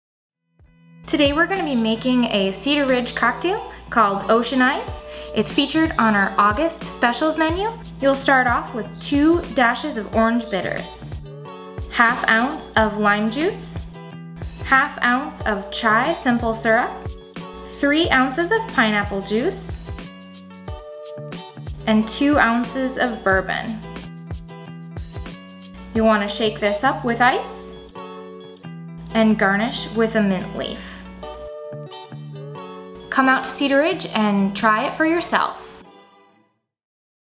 Ocean Eyes | Cocktail Creations
A delicious summer drink that is full of flavor and uses our Cedar Ridge Iowa Bourbon! This drink is featured on our August Specials menu, and is called Ocean Eyes. Swing on by this month to give it a try! Cheers!